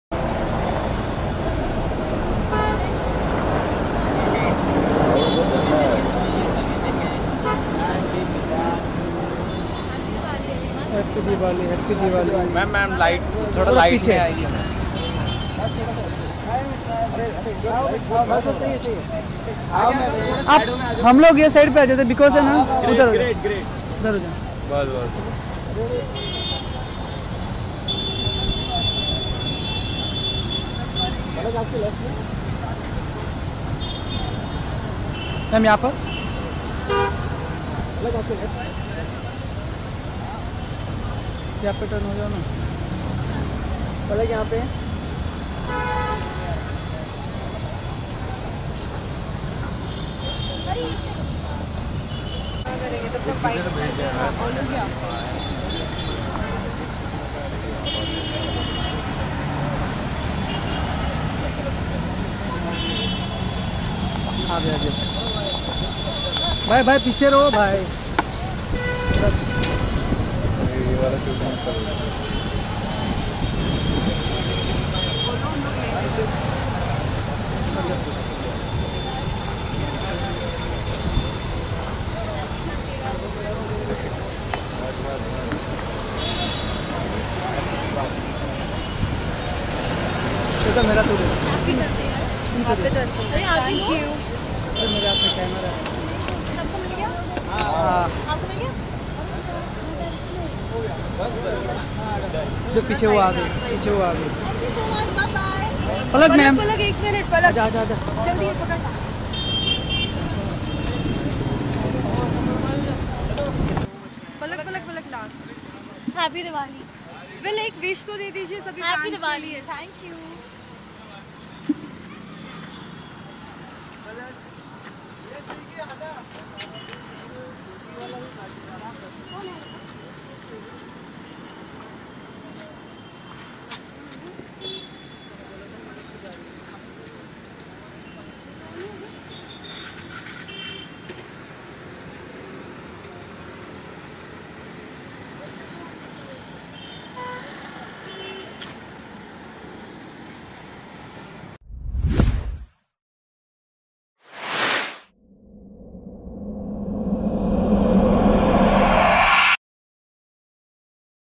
Palak Tiwari ने बांटी Media को Diwali की मिठाई!!! Palak Tiwari Snapped At Andheri
Watch Palak Tiwari ने बांटी Media को Diwali की मिठाई!!! Palak Tiwari Snapped At  Andheri